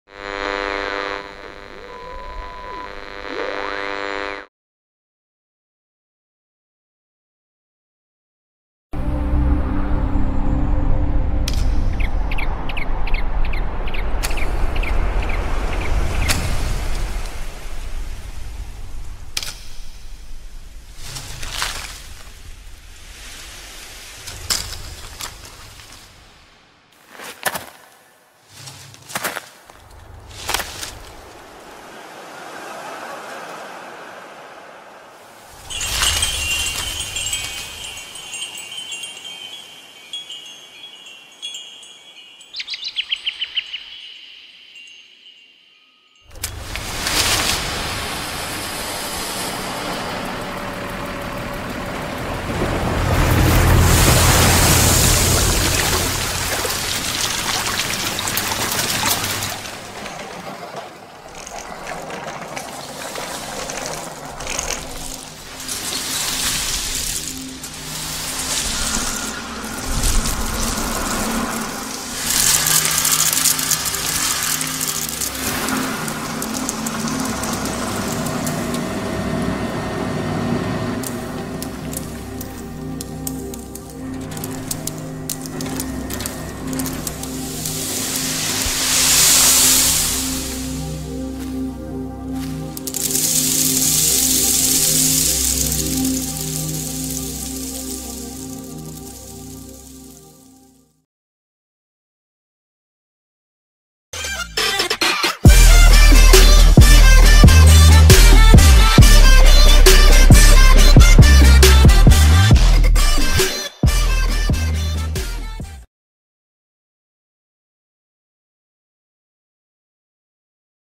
Iranian Green Gold - Harvesting & Processing Just in One minute - پسته ، طلای سبز ایران
The pistachio tree is an old tree with a great antiquity. There are many opinions about its origin; mostly know the internal area of IRAN as the original region that this tree has grown there for the first time. The historic signs about its antiquity show that thousand years before Christ its nut has been eaten as a food in Asia. After many years it introduced and carried to some other parts of world.